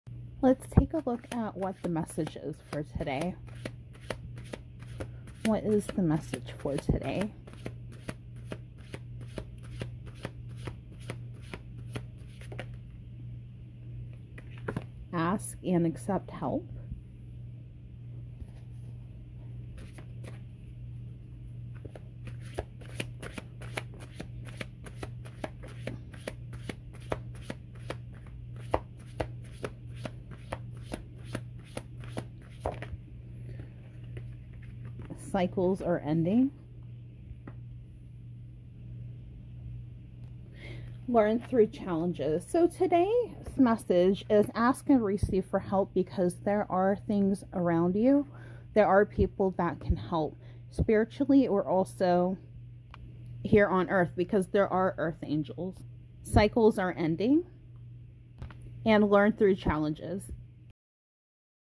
Oracle Card Reading for Spiritual Guidance || Message of the Day @HealingDaysOracle
Oracle cards have a message for you each day. What can those messages be telling you? In this video, I'll read the cards and offer some insights into what they might mean for your future.

NOW TAKING PEOPLE FOR PERSONAL READINGS.

"Soft and sweet, the Plushies N Coffee shop is coming soon." Sign up to be the first to know when the shop goes live.